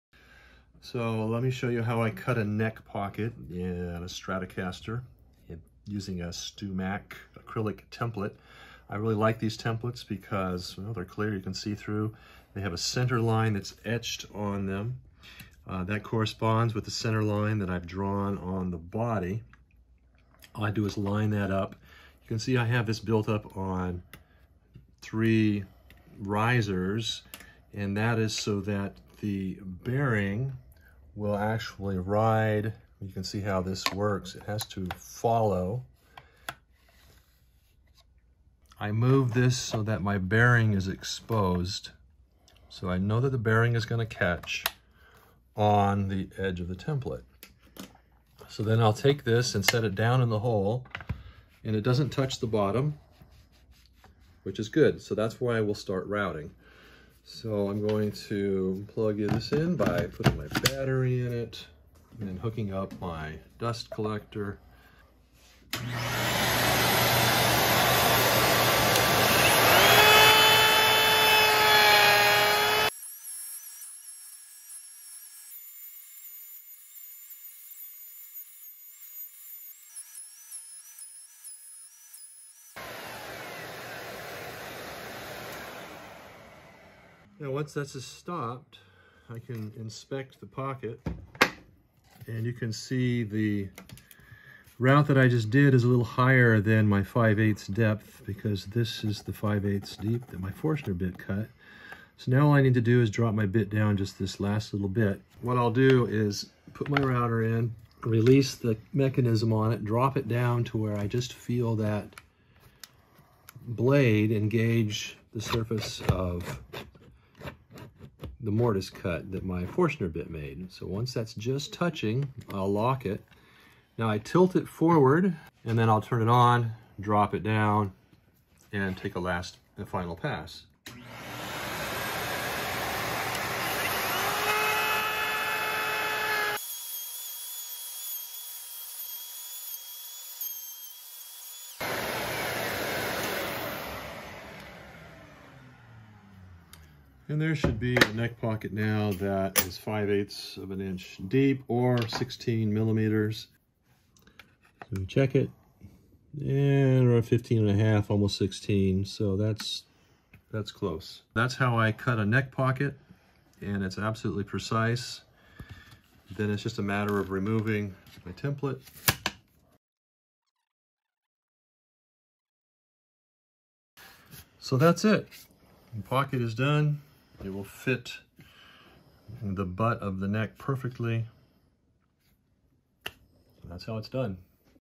Routing a Strat Style Guitar Neck Pocket
This is how I rout the pocket. Easily done using a template and a router.